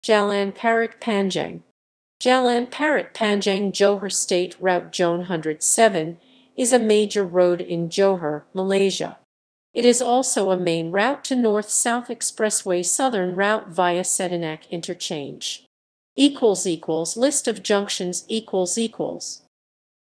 Jalan Parit Panjang
Jalan Parit Panjang (Johor state route J107) is a major road in Johor, Malaysia. It is also a main route to North–South Expressway Southern Route via Sedenak Interchange.

== List of junctions ==